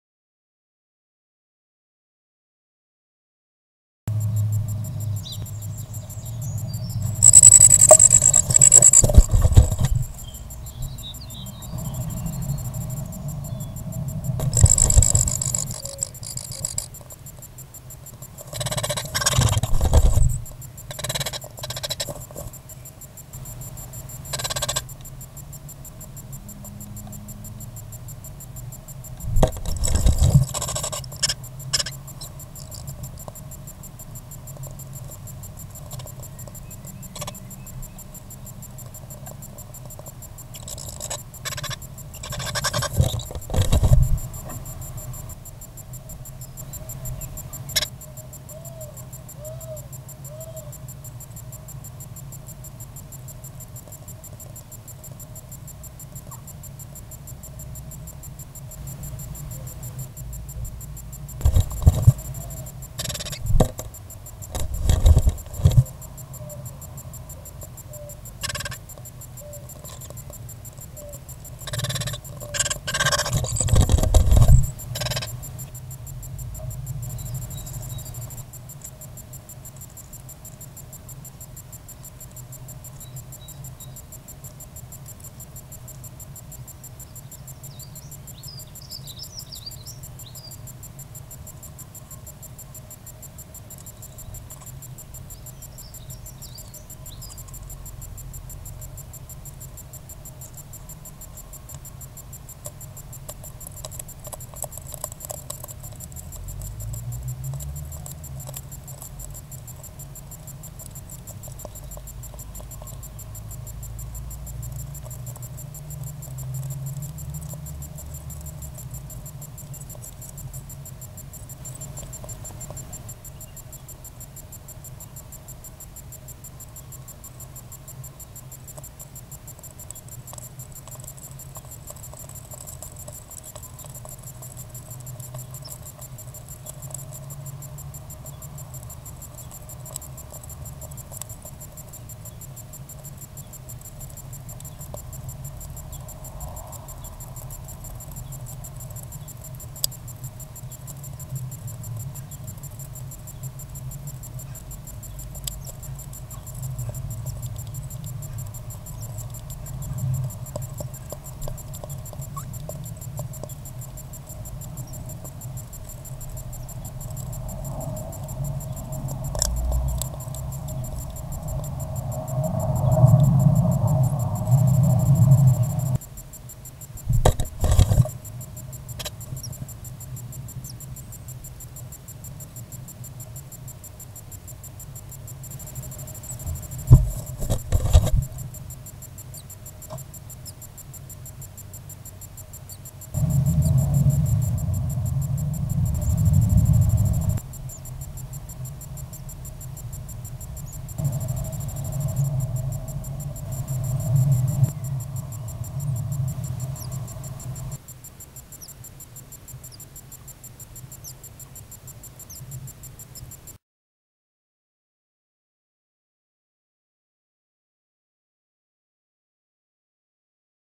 Great Tit hatching day
great tits hatching with parent feeding and shell eating